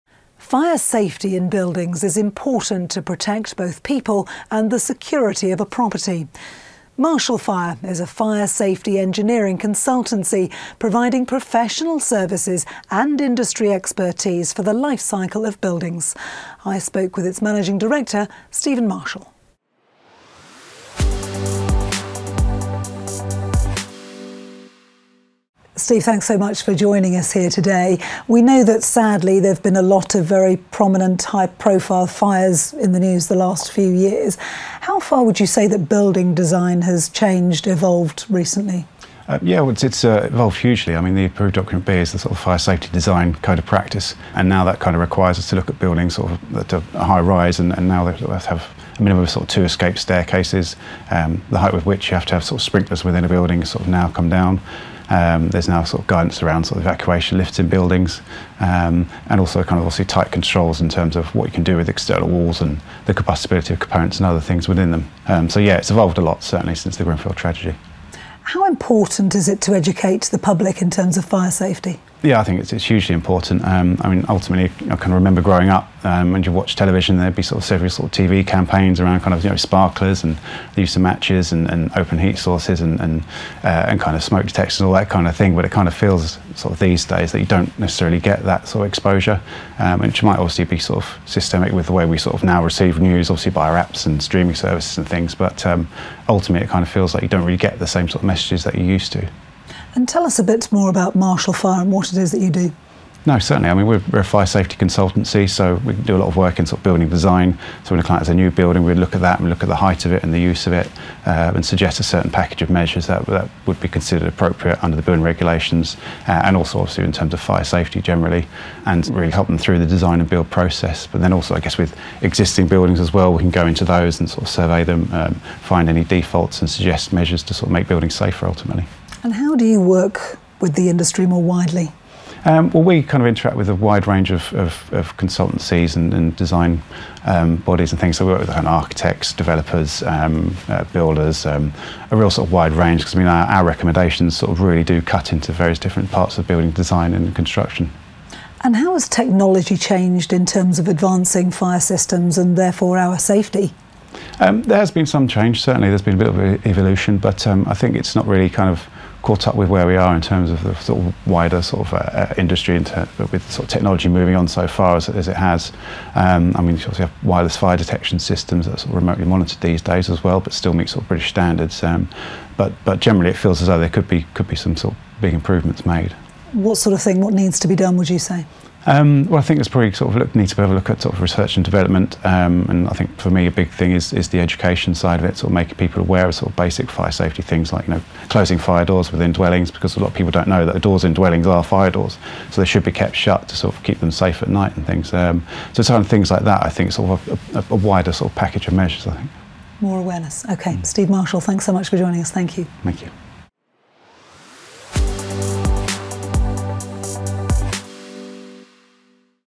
The lifecycle of buildings
Fire safety in buildings is important to protect both people and the security of a property. Marshall Fire is a fire safety engineering consultancy providing professional services and industry expertise for the lifecycle of buildings.

For more content on fire safety, technology and innovation, as well as hearing from the voices of key opinion leaders, individuals and organisations across the world of business, visit the ITN Business Hub